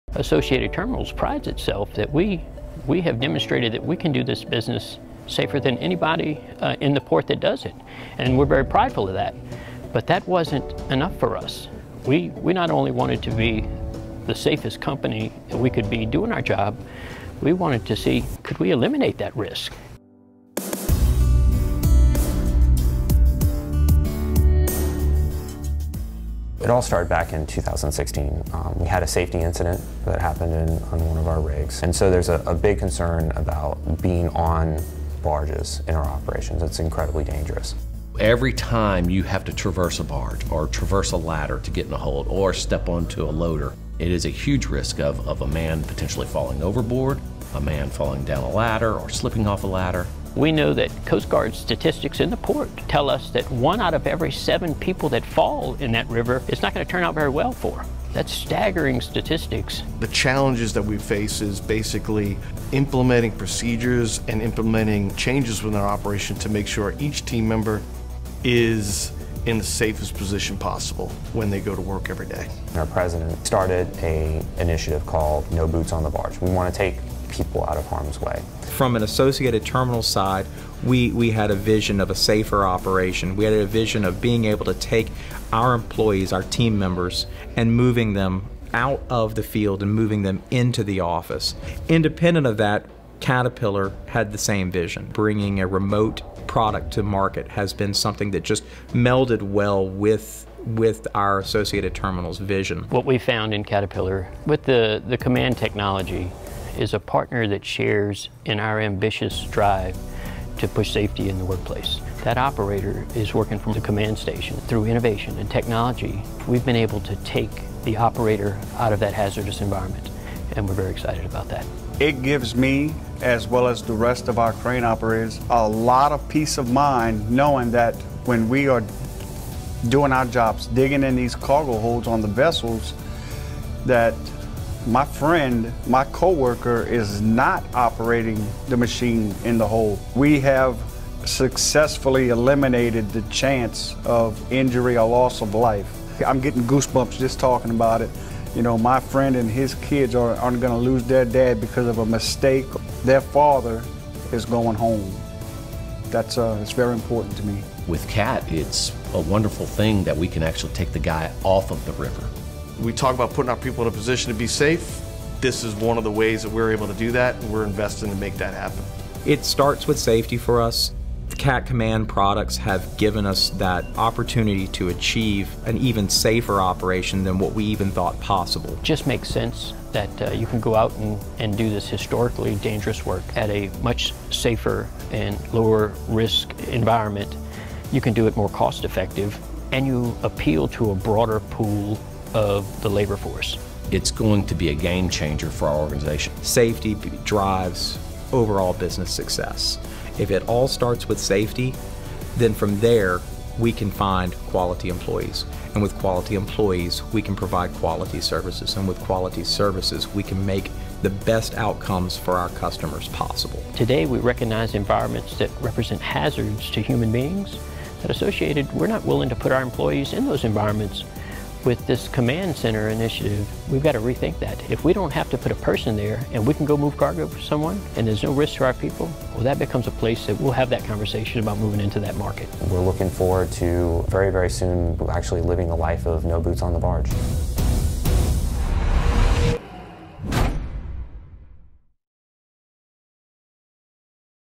Cat® Command – Safety
Listen to Associated Terminals discuss how the Cat® Command technology is vital to the safety of their operation.  They strive for innovative ways to keep their employees safe and out of harm’s way.  Cat Command technology is helping them to achieve a “no boots on the barge” initiative.